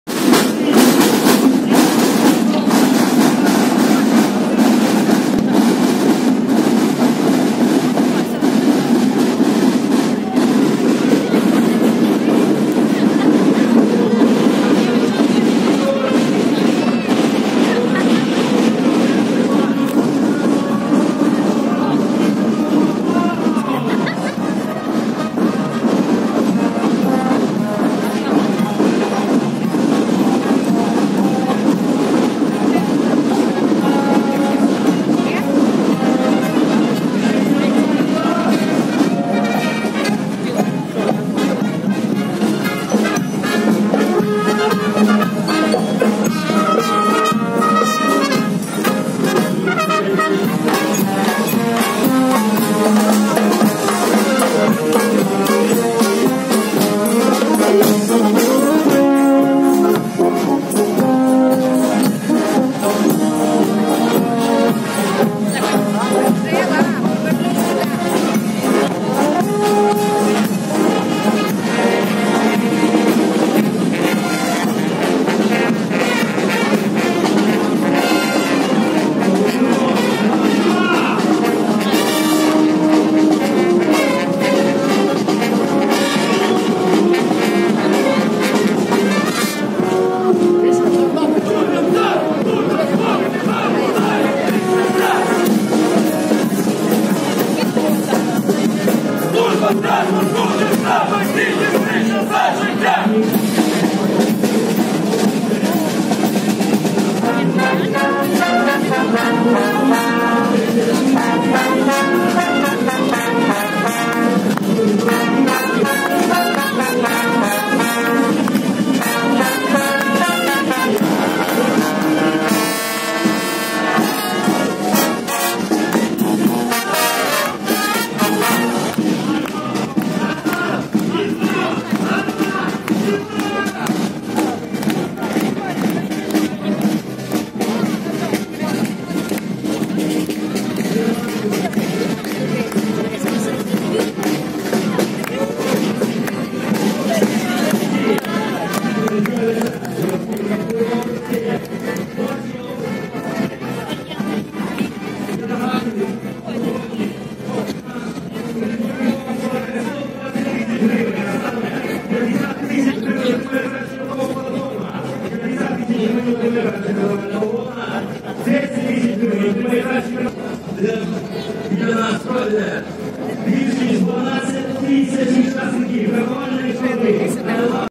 Дніпро. День міста.три роки тому @-sunflowers
День міста пройшов непогано. народ "зголоднів" за парадами. Хліба та видовищ було дотримано.
 На параді стільки було мотоциклістів, що народ мало не задихнувся від вихлопів, а яка чудова реклама була СМАРТ!
 А ввечері, деякі матусі о 22 годині навіть забули про те, що діти до 16 років повинні лягати спати о 21.00, яке там спати, діти з батьками, дивляться, як батьки випивають. ... відірвалися!